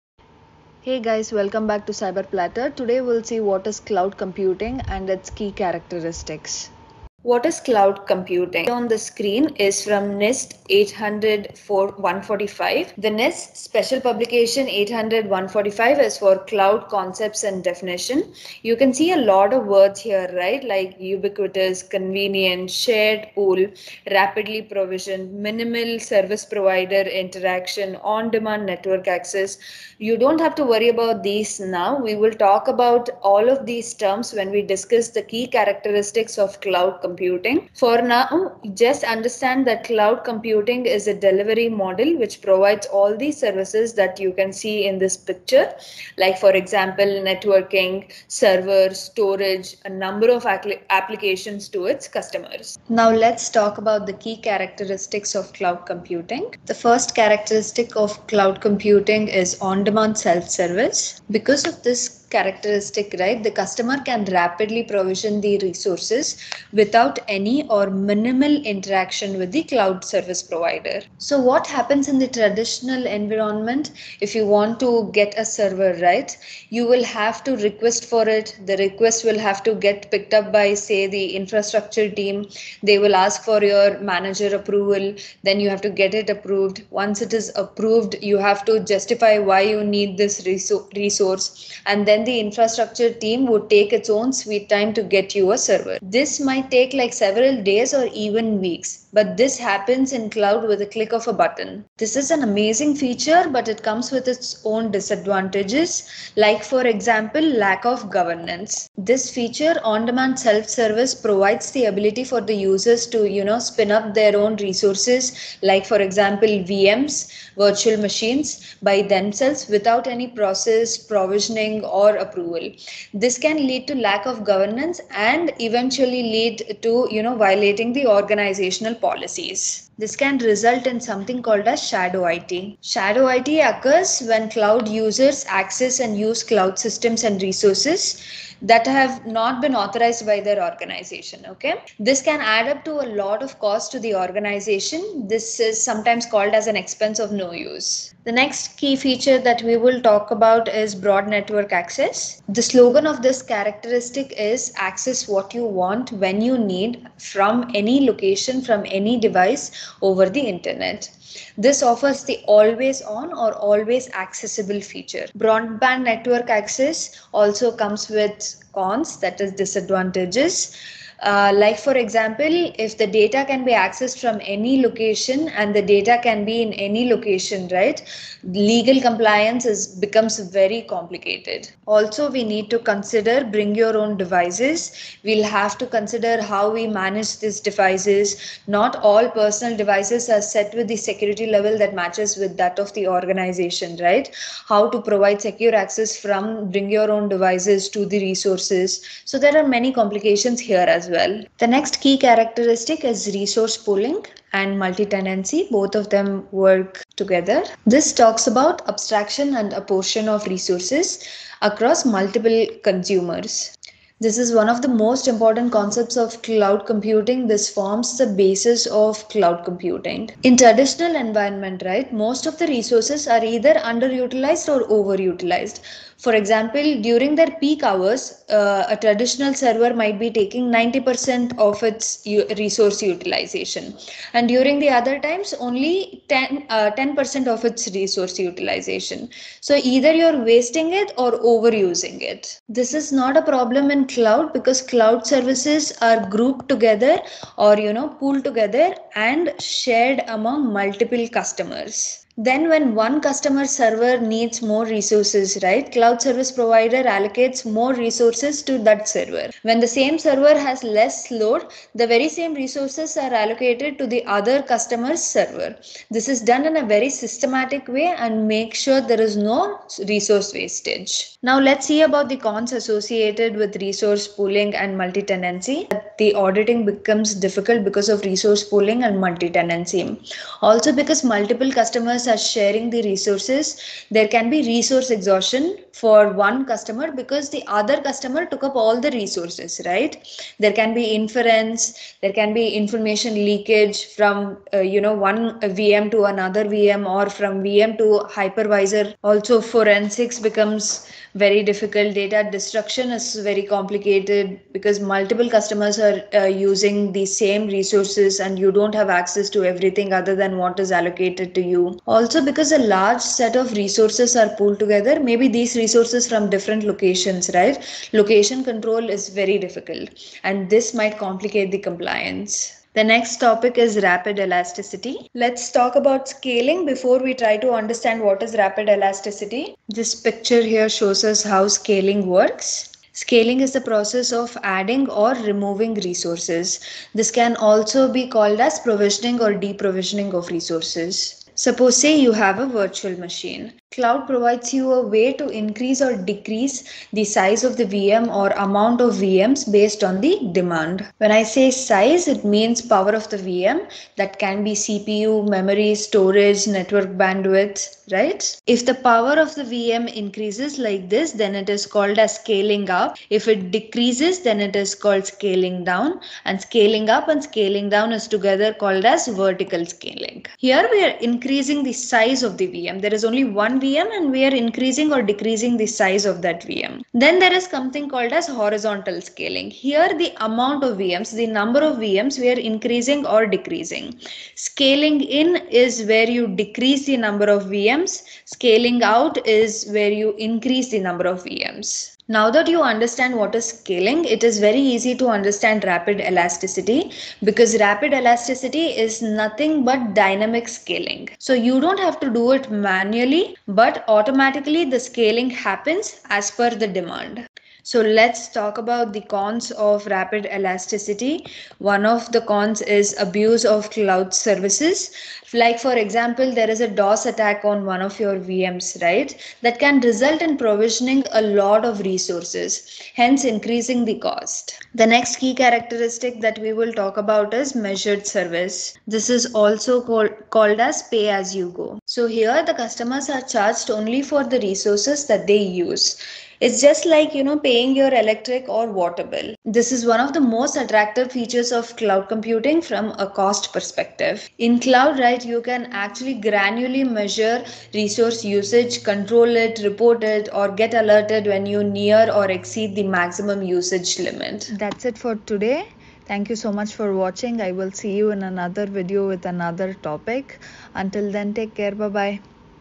CCSP | Cloud Computing | Key Characteristics of Cloud Computing | Pros and Cons of Cloud Computing
This video includes:
            What are the key characteristics of cloud computing?
                        On demand self service
                        Broad Network Access
                        Resource Pooling | Multitenancy
                        Rapid Elasticity | Scaling
                        Measured Service | Pay as you go